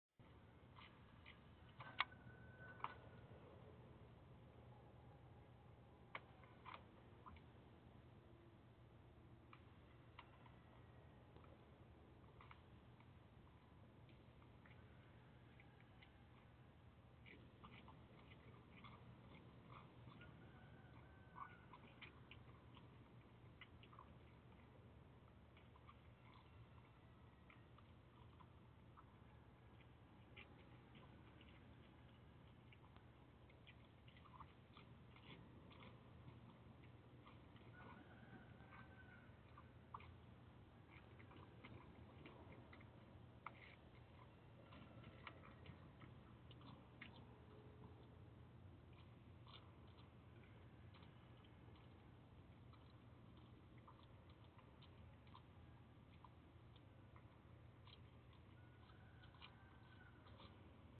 baby javelinas with mom
the javelina had babies! brought them to the water dishes